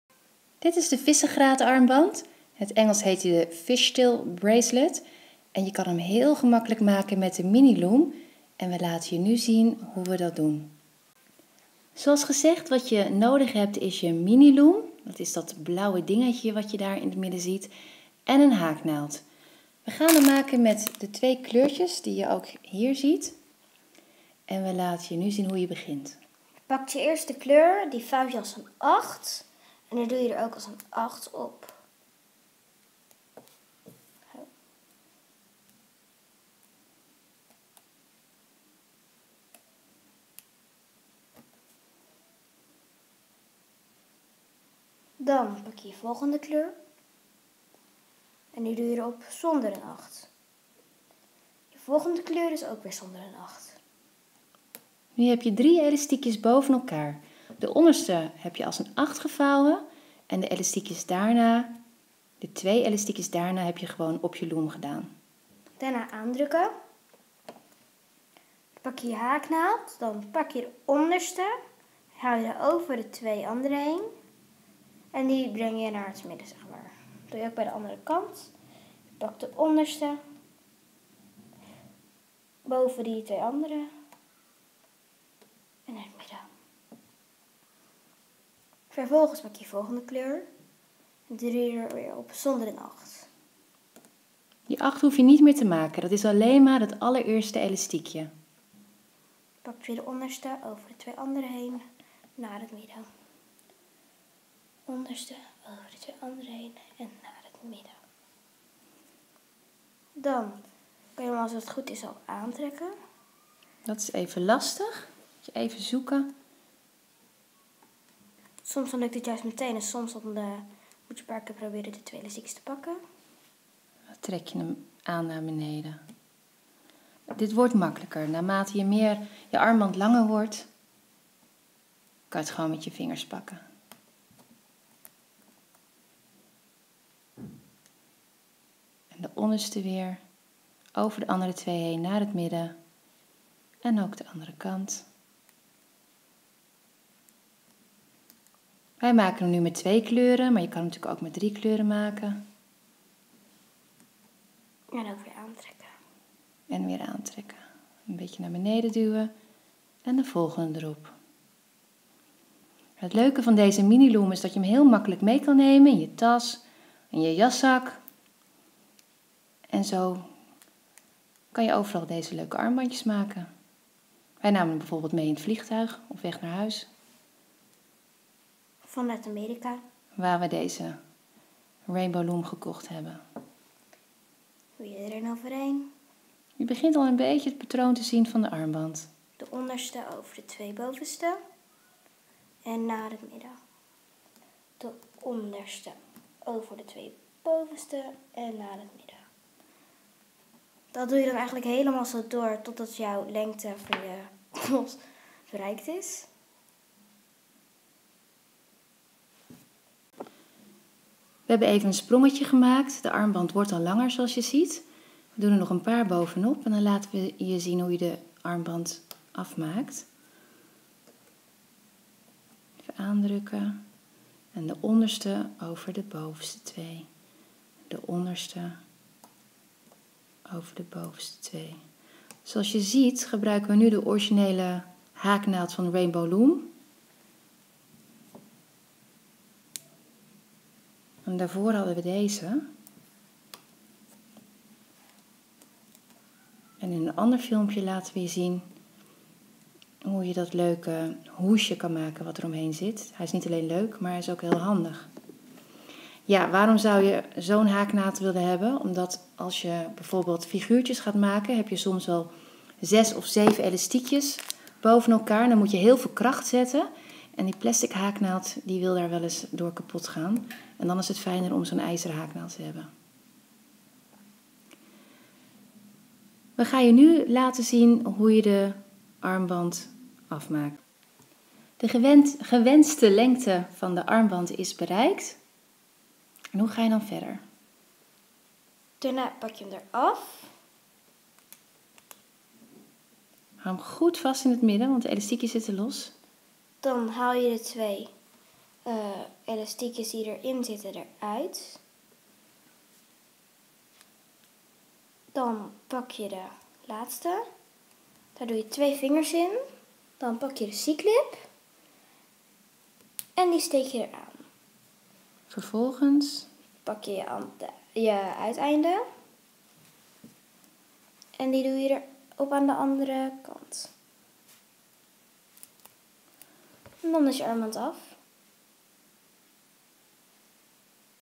Rainbow Loom, Nederlands, fishtail, vissenstaart, visgraat, armband, gemaakt met mini loom
Erg leuk en gemakkelijk te maken armbandje. Hier zie je hoe we het maken met de mini loom. Je kan deze armband doorhaken tot elke lengte die je wil waardoor je geen aparte verlenging nodig hebt.